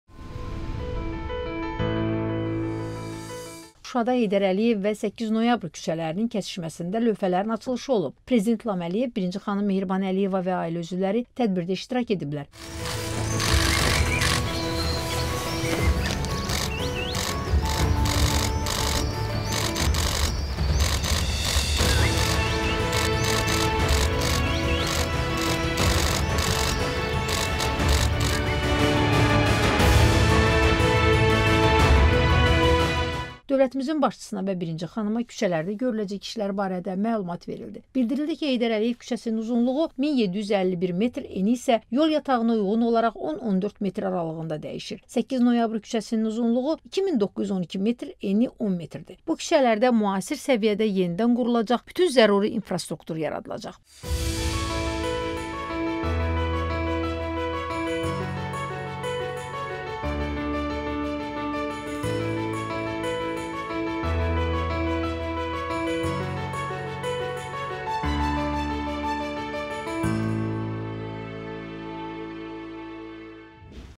Şuşada Heydər Əliyev və 8 Noyabr küçələrinin kəsişməsində lövhələrin açılışı olub
Mayın 10-da Şuşada Heydər Əliyev və 8 Noyabr küçələrinin kəsişməsində lövhələrin açılışı olub.
AZƏRTAC xəbər verir ki, Azərbaycan Respublikasının Prezidenti İlham Əliyev, birinci xanım Mehriban Əliyeva və ailə üzvləri tədbirdə iştirak ediblər.